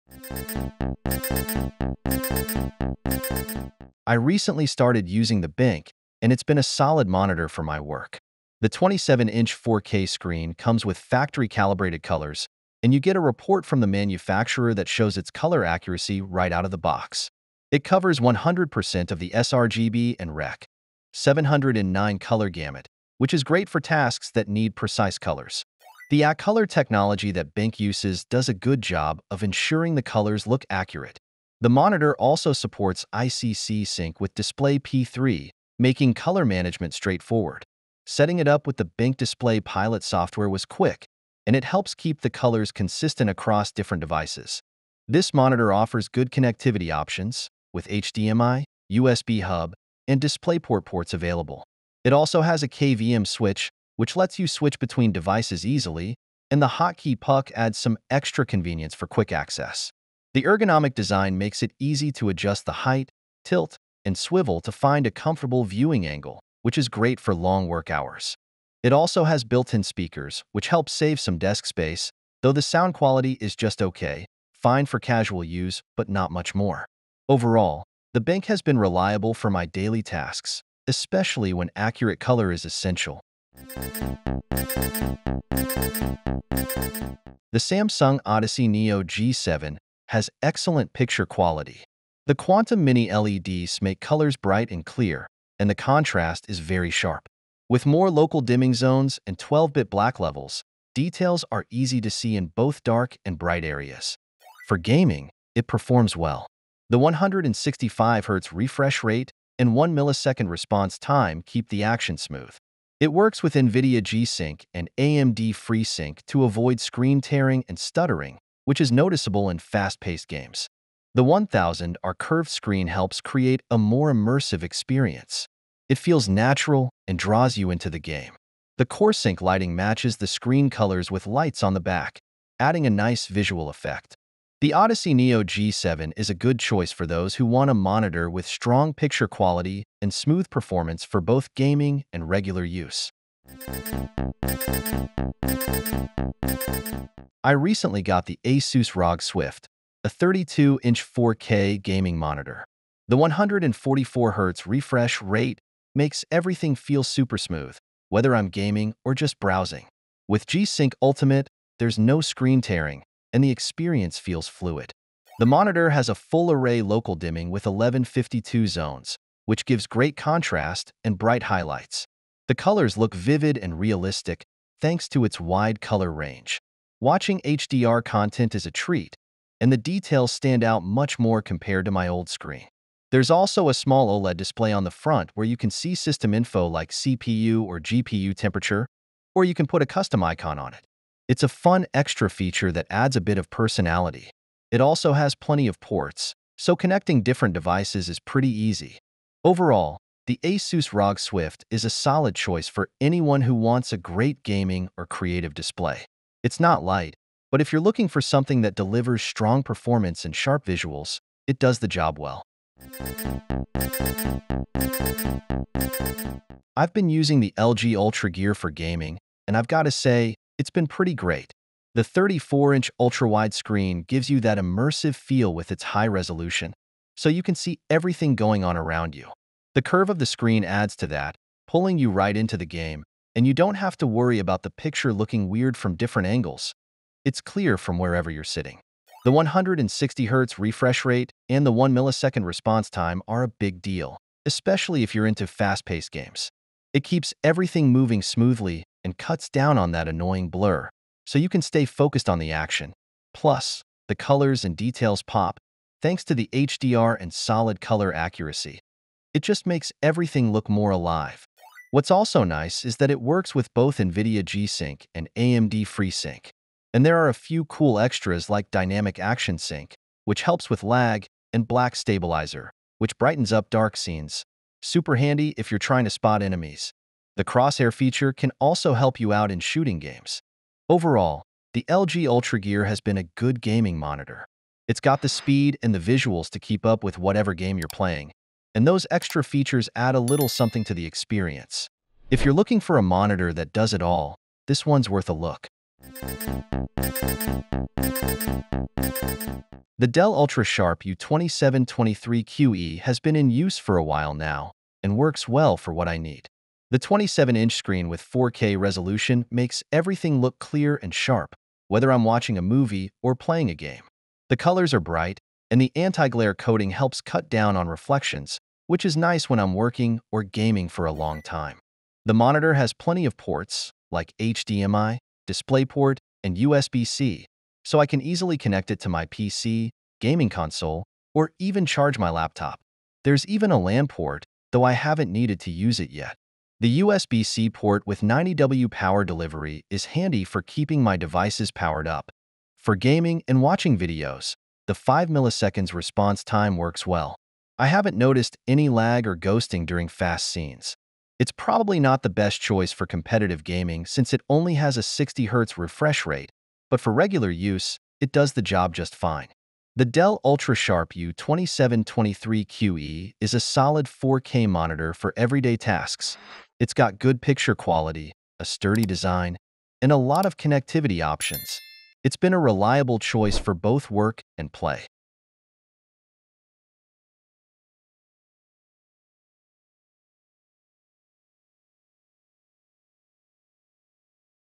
Top 5 Monitors of 2024: Find Out Which One Tops the List
5th place: BenQ PD2705U
4th place: Odyssey Neo G7
3rd place: ASUS ROG Swift PG32UQX
2nd place: LG UltraGear 34GP83A-B
1st place: Dell UltraSharp U2723QE

Discover the top 5 monitors of 2024, ranked for their performance, features, and value. From BenQ's exceptional color accuracy to Dell's ultra-sharp display, find out which screens are best for gaming, creative work, or professional use. Dive into this ultimate guide and upgrade your setup today!

00:00 BenQ PD2705U
01:23 Odyssey Neo G7
02:23 ASUS ROG Swift PG32UQX
03:32 LG UltraGear 34GP83A-B
04:55 Dell UltraSharp U2723QE